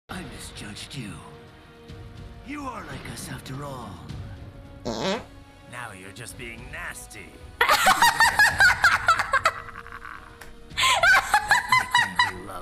My toots got called out
A lil sneak peak of Fart Comp 7! This was my first time playing; this toot was not scripted!